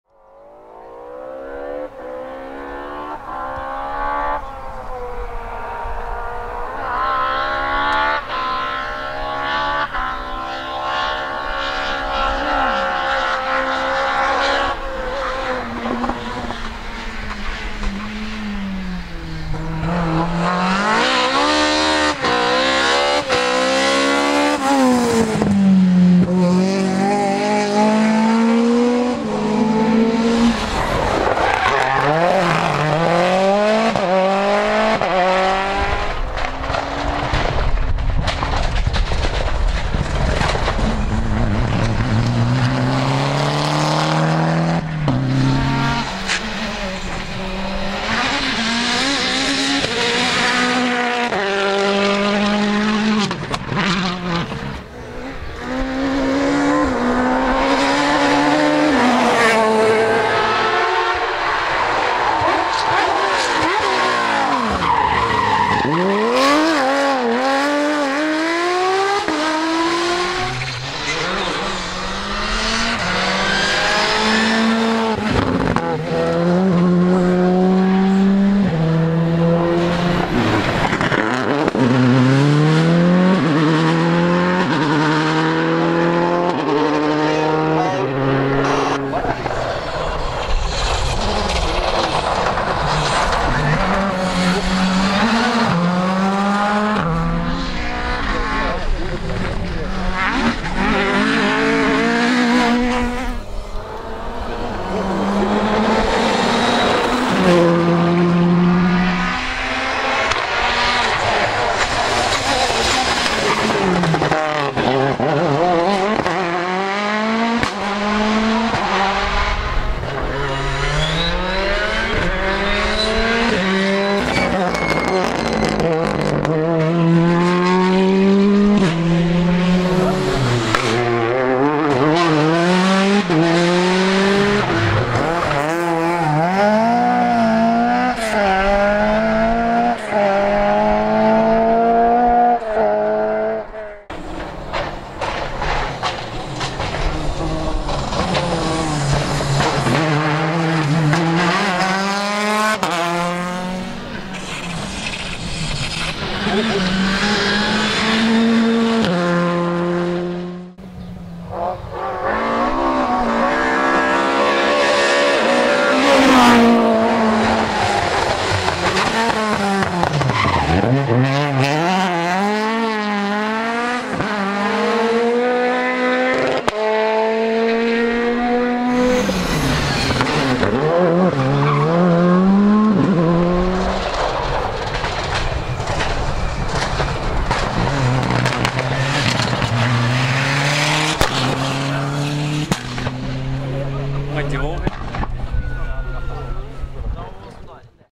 Promo VAS Rally van Wervik 2019 by ProRallyVids
Promo for the upcoming Rally van Wervik 2019. Video's are from edition 2018.